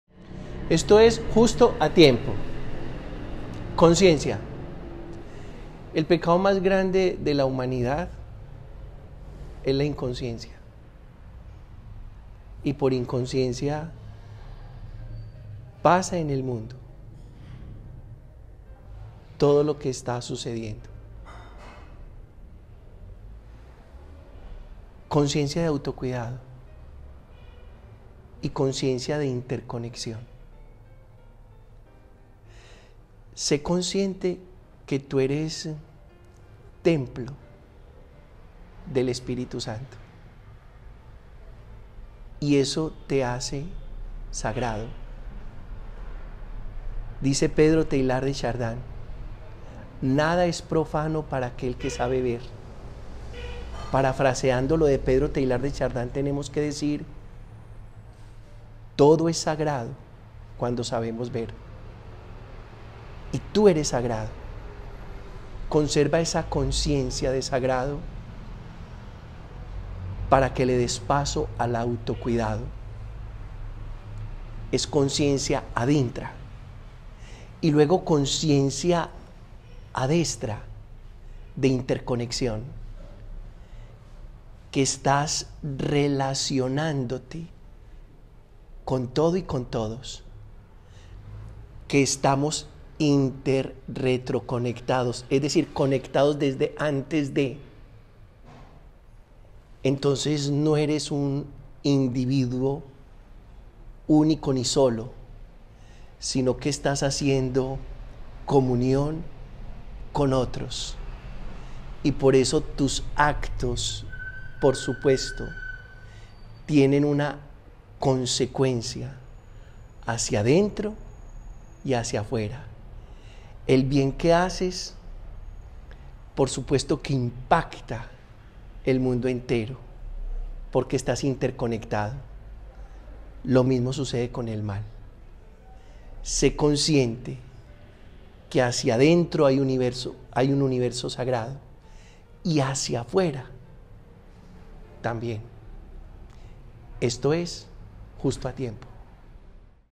Me dice la conciencia, Padre Pedro Justo Berrío 🤝 Justo en amor y amistad - Tele VID
El Padre Pedro Justo Berrío comparte con nosotros un mensaje para que vivamos de la mejor forma del AmorYLaAmistad.